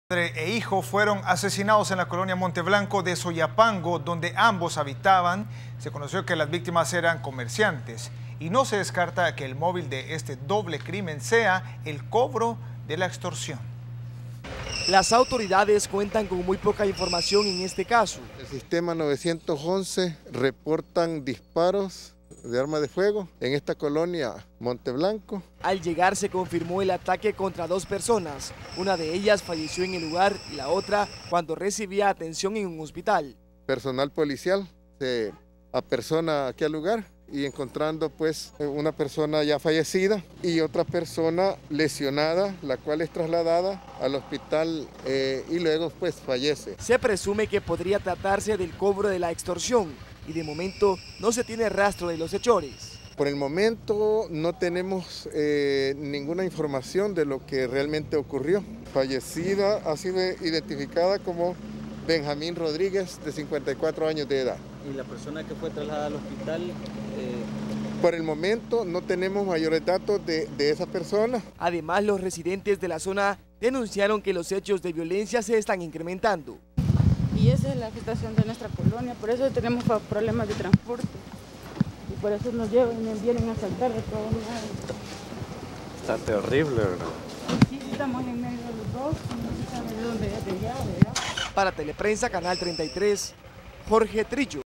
Asesinan a padre e hijo en Soyapango
Padre e hijo fueron asesinado en la Colonia Monte Blanco de Soyapango, donde ambos habitaban.
Se conoció que las víctimas eran comerciantes y no se descarta que el móvil de este doble crimen sea el cobro de la extorsión.